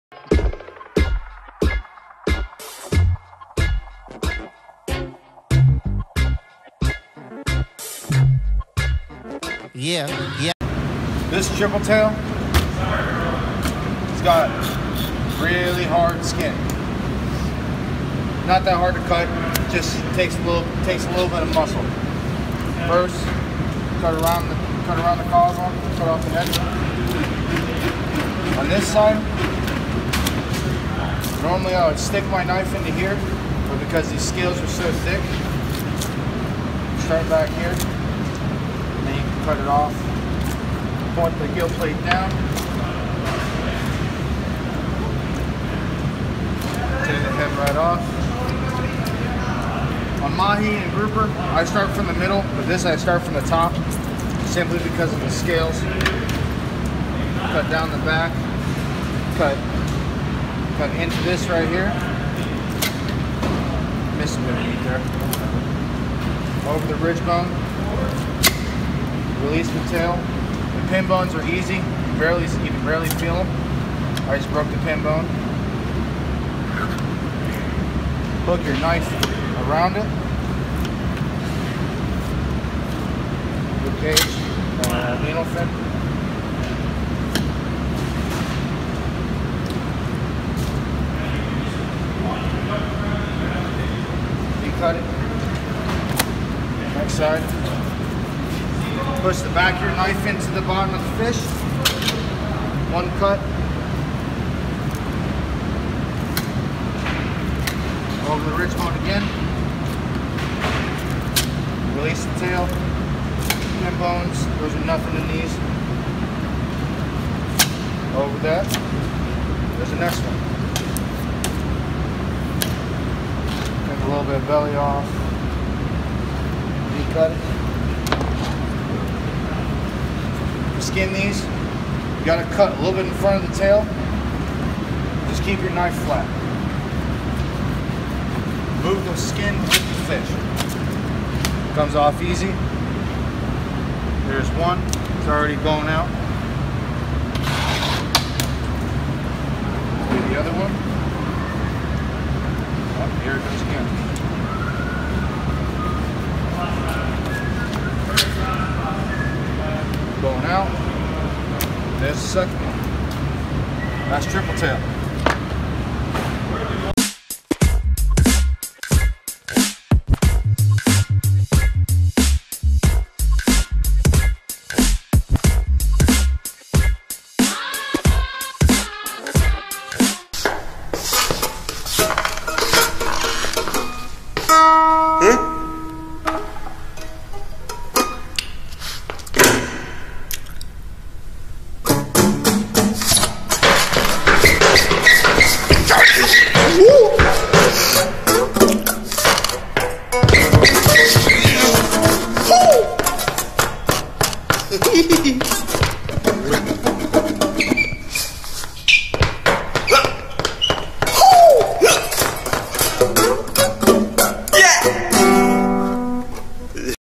How to Fillet Fish: Triple Tail[Takes Just A little bit of Muscle] Easy and quick, Cleaning Fish
Triple Tail is not difficult to cut but its tough and unforgiving. These fish are extremely aggressive and strong for their size. Also, they are very hard to catch.

For the Elvis bit
all rights by Paramount Pictures except the sound design.

-created for parody purposes.